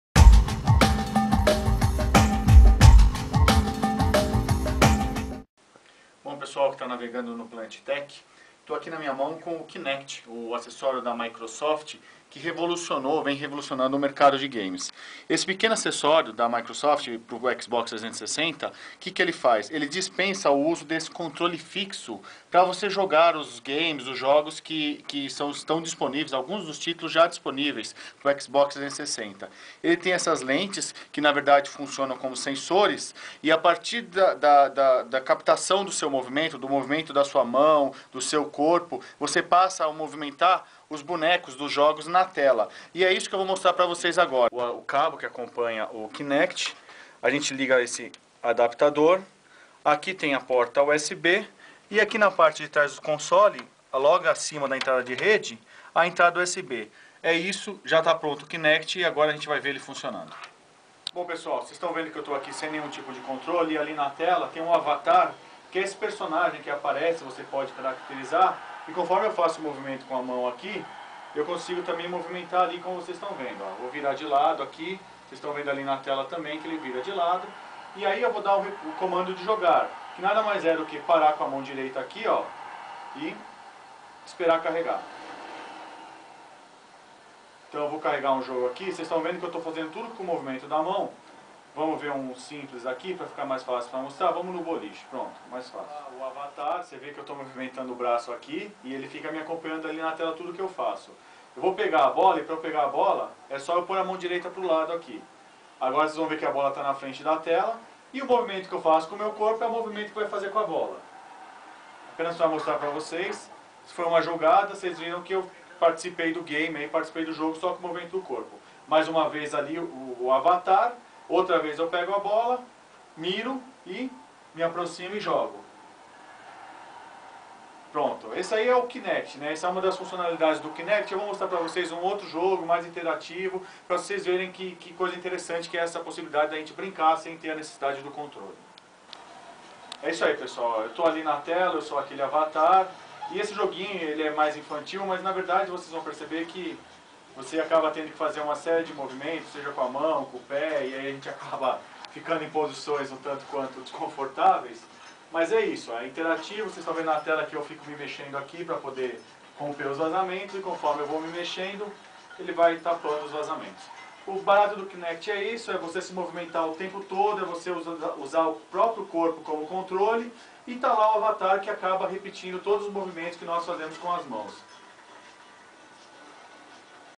Kinect vende 10 milhões e entra para o Guinness
A Microsoft divulgou um comunicado ontem relatando que comercializou, em apenas quatro meses, 10 milhões de unidades do Kinect, o sensor de movimentos do console Xbox 360.

A expressiva marca superou até mesmo o iPad, que se transformou numa verdadeira febre em todo o mundo, e demorou nove meses para ultrapassar a casa dos 15 milhões.

Esses números levaram acessório para o livro Guinness World Records, como o eletrônico que vendeu mais rapidamente na história. Lançado em novembro do ano passado, o Kinect vendeu uma média de 133 unidades por dia.

A Microsoft afirmou ainda que mais de dez milhões de games para o sensor de movimentos foram comercializados neste período.

O vídeo acima mostra o principal motivo do Kinect  ter alcançado esses números.